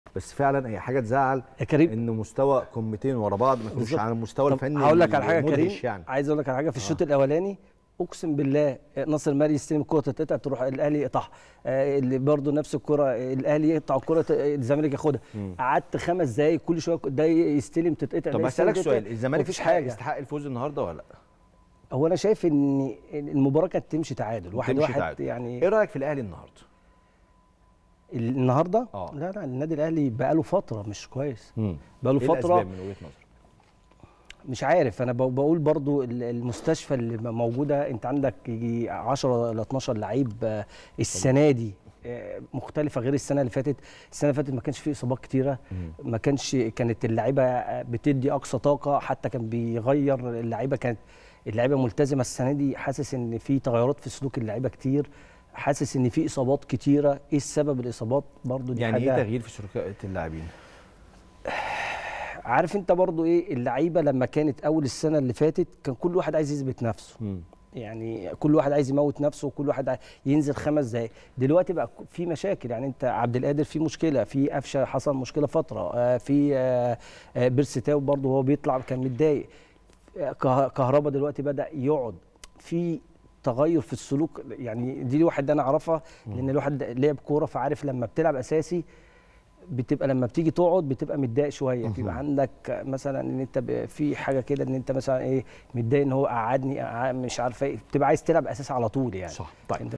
🔥🔥تعليق مفاجئ من حسن مصطفى على اداء الأهلي والزمالك:اوكان المفروض المباراة تنتهي تعادل🔥💣
🔥🔥تعليق مفاجئ من حسن مصطفى على اداء الأهلي والزمالك:الزمالك لم يستحق الفوز وكان المفروض المباراة تنتهي تعادل🔥💣